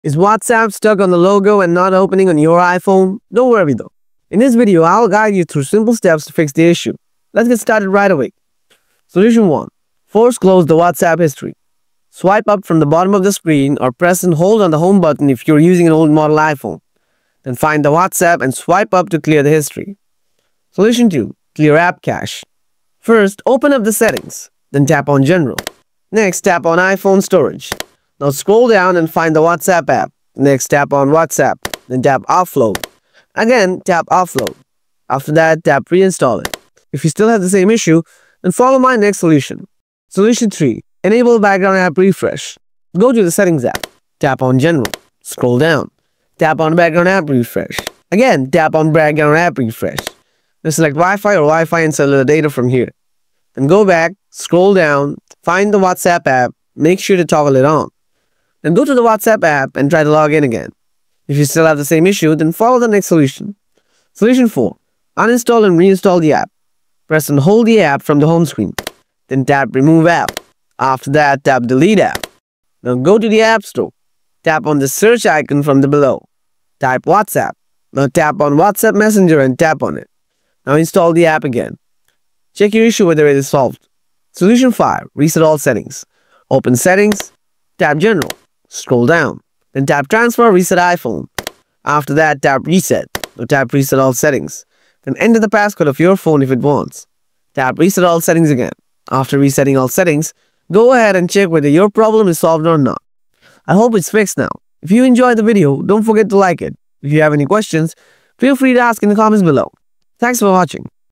How to Fix WhatsApp Not Opening and Stuck on Its Logo | Resolve WhatsApp Not Working Issue
Wanna know how to fix WhatsApp when it’s not opening or stuck on the logo? Don't worry! If WhatsApp is freezing on the home screen or not working at all, I've got you covered! In this video, I’ll show you exactly how to fix WhatsApp not opening and how to resolve the issue when WhatsApp is stuck on its logo. I’ll also dive into why WhatsApp gets stuck and how to troubleshoot and fix WhatsApp not working on your device. Stick around to learn all the quick fixes for your WhatsApp freeze problem. Let's get started.

00:01- Video Intro
00:09- Solution 1: Force Close WhatsApp History
00:20- Solution 2: Clear App Cache
00:40- Solution 3: Enable Background App Refresh
01:04- Solution 4: Uninstall and Install App
01:24- Solution 5: Reset All Settings
01:39- Solution End